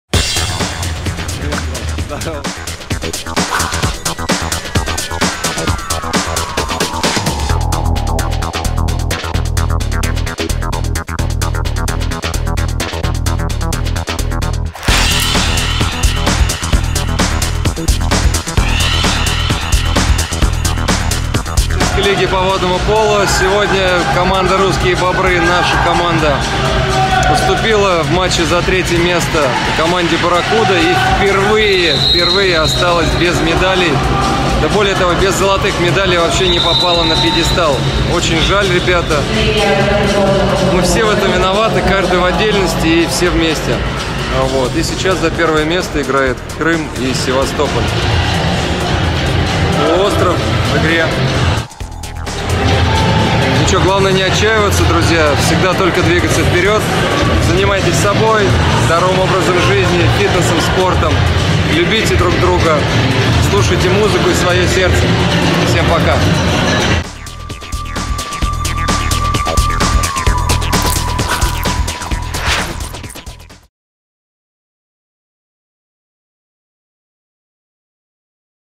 Финал НВА В этом году RB без медалей. Но мы вернемся!
Впервые в истории Russian Beavers остались без золота и вообще без медалей. Это все в копилку опыта! Успехов!